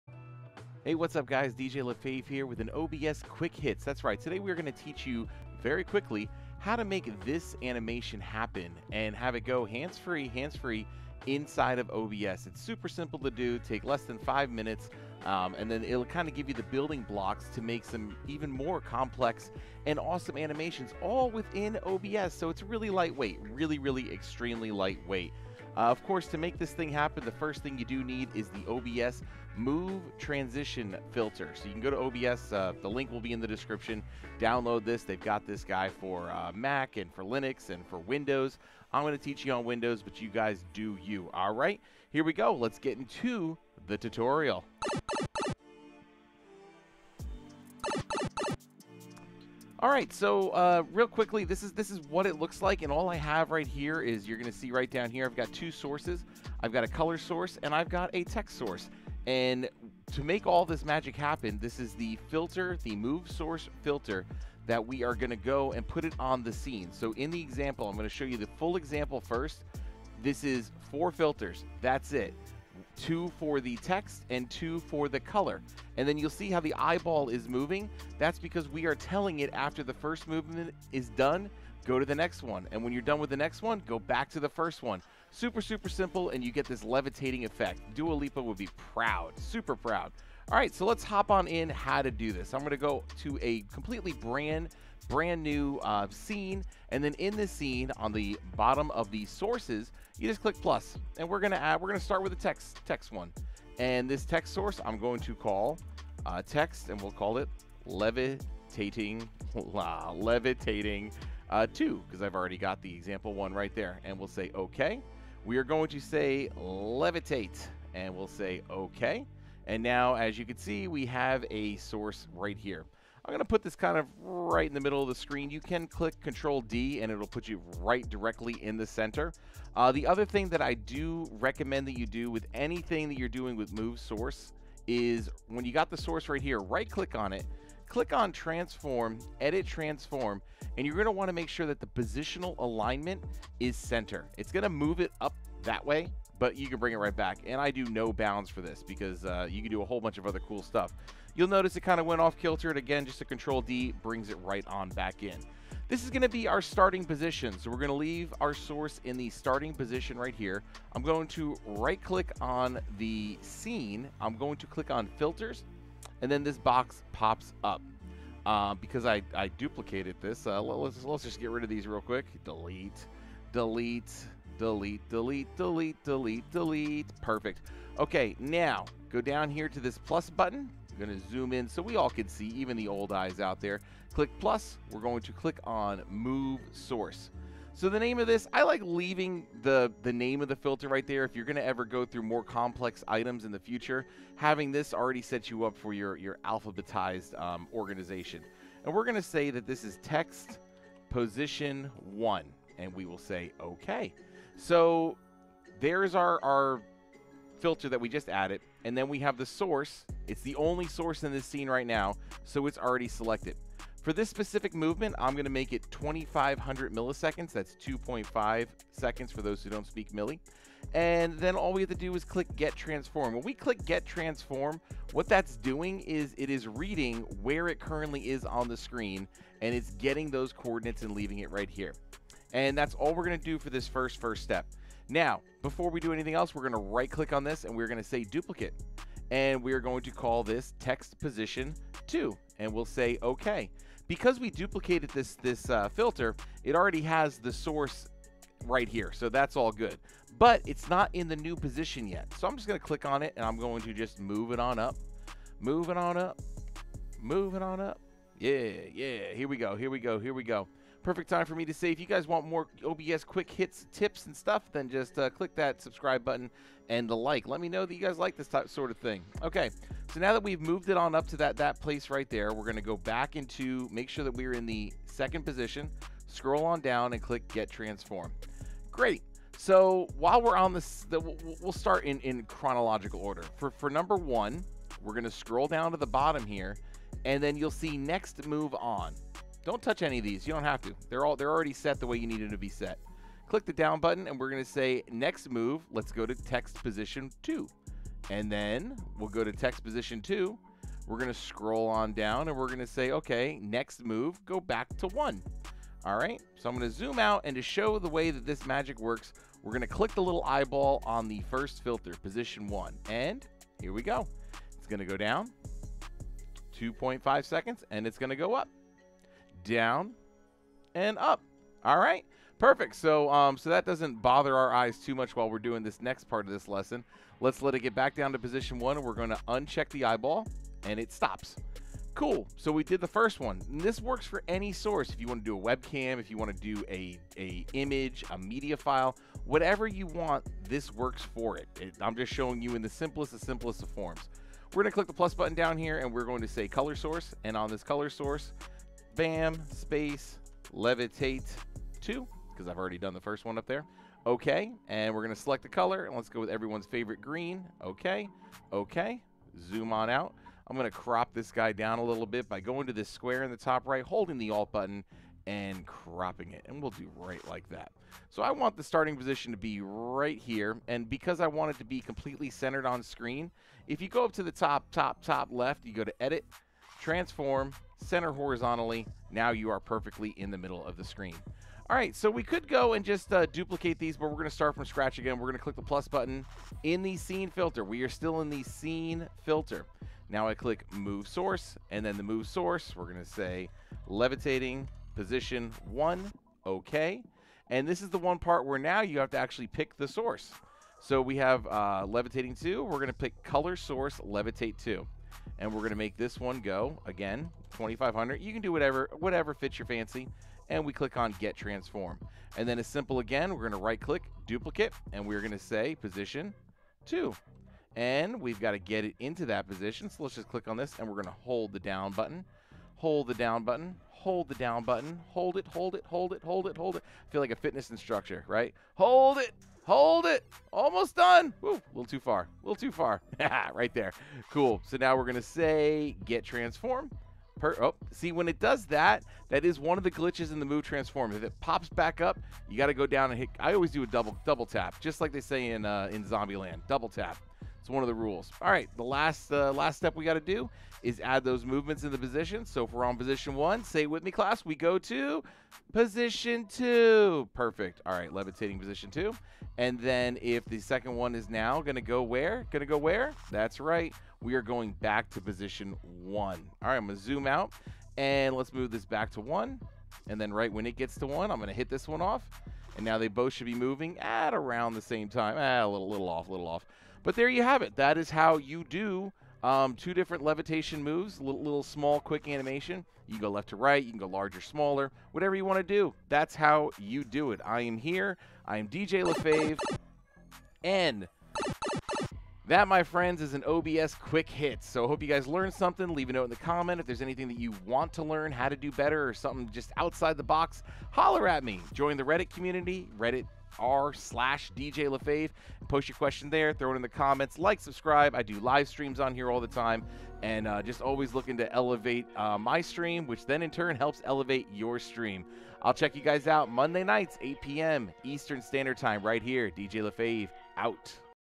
Easily Animate a Source in OBS Studio
🎬 **About This Video:** Making animations inside of OBS has never been easier with the Move Transition Plugin. This OBS Quick Hit tutorial will show you how to animate two different sources to appear like they are levitating. It’s super simple and hopefully this lesson will help spark some creative ideas to help elevate your stream!

• Time Stamps •
00:00 - Intro
00:30 - What you Need
00:45 - Example Showcase (The Why/How it Works)
01:30 - Realtime Walkthrough Begins (Animating Source 1)
05:44 - Realtime Walkthrough Continues (Animating Source 2)
08:05 - Warping things up and reminding you to Like and Subscribe